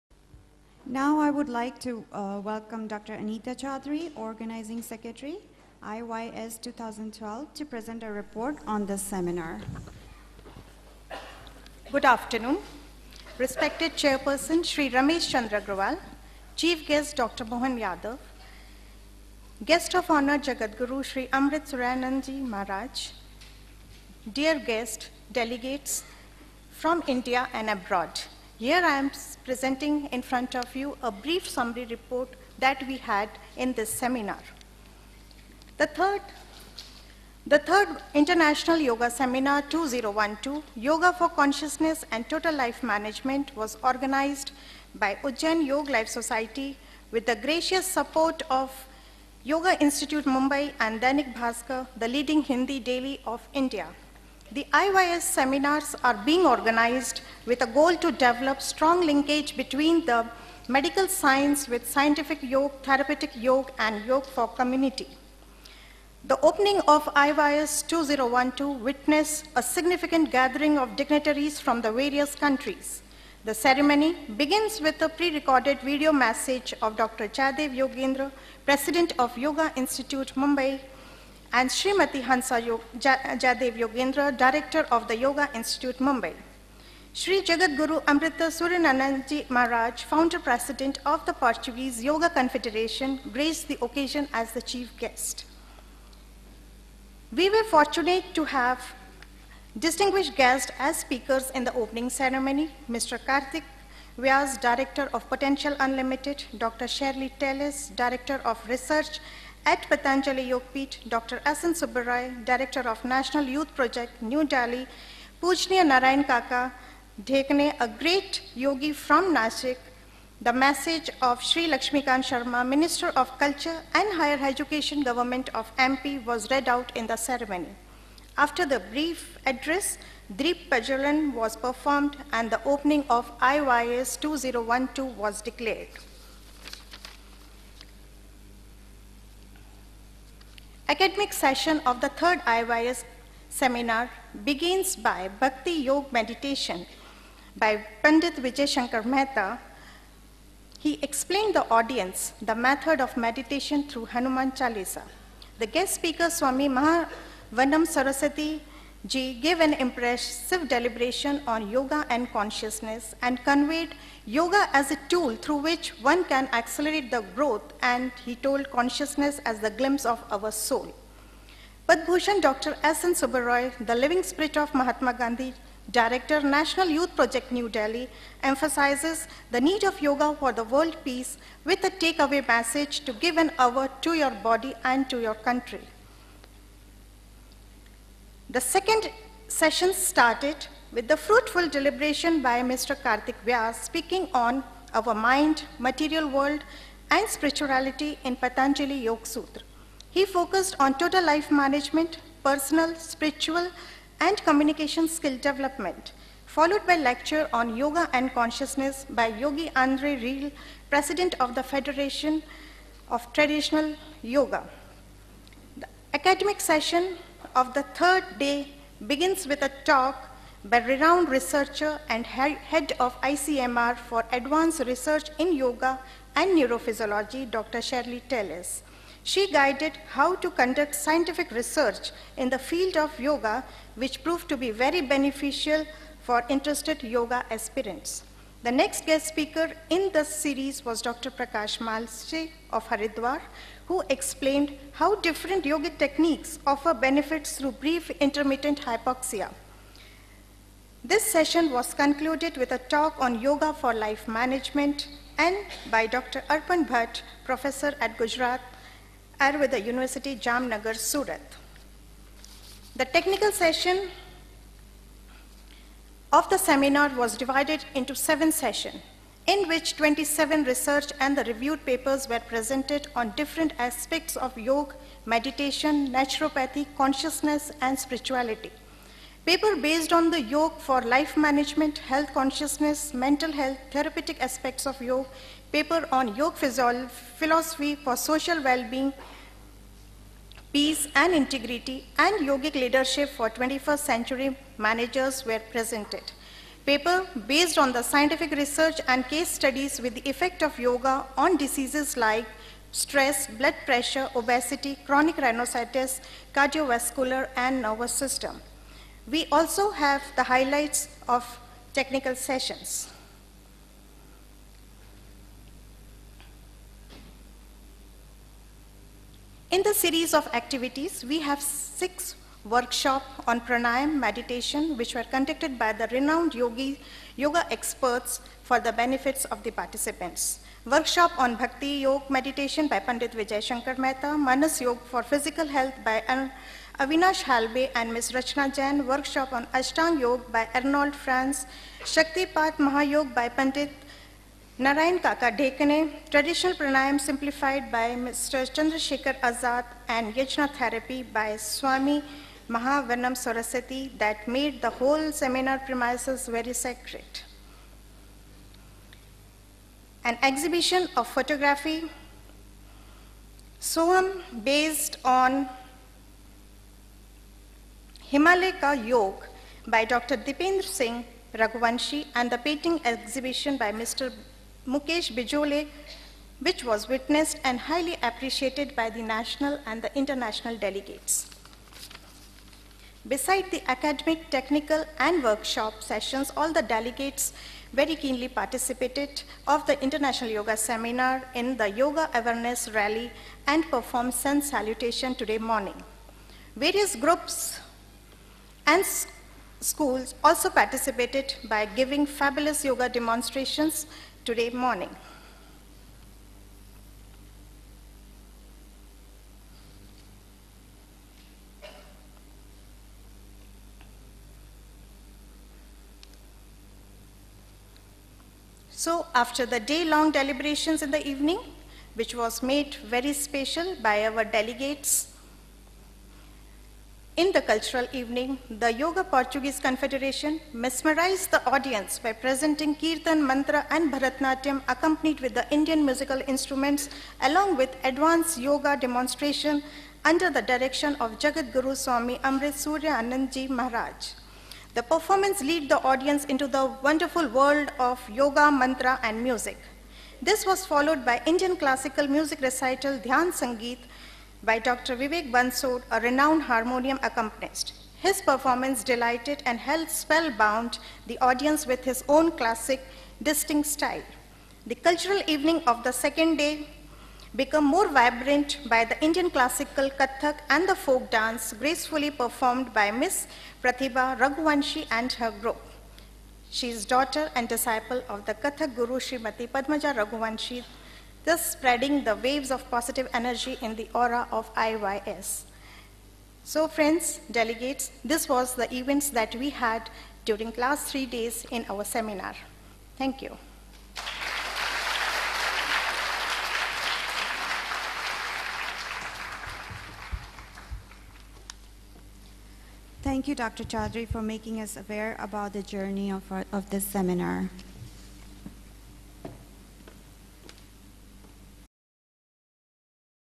Dr Anita Choudhary, Organising Secretary of IYS-2012
Presenting Report of 3rd International Yoga Seminar, 12-14 October, 2012, Ujjain. India.
in Closing Ceremony of IYS on 14th OCtober 2012.